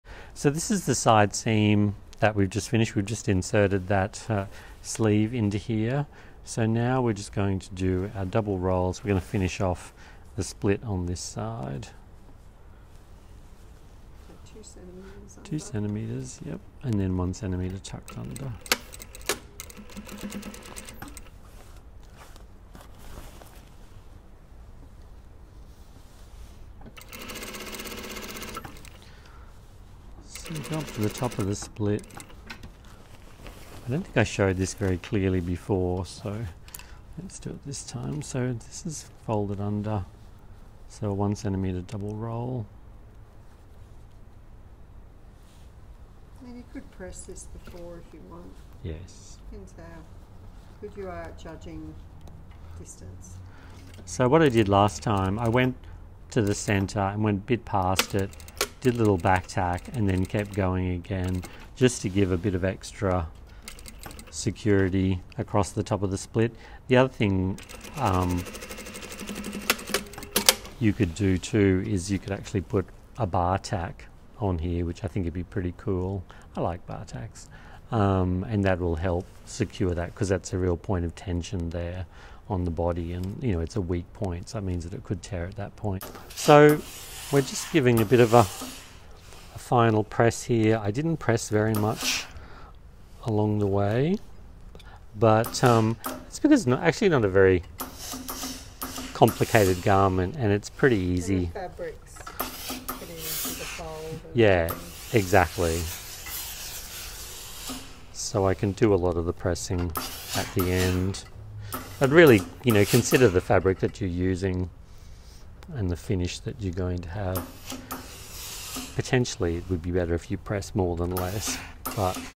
Part 18: 1st year blouse exercise - RHS finish
Instructions for finishing the right-hand-side of the garment, including finishing the side split with a double turned hem (1cm finish) and ironing the garment using the industrial iron.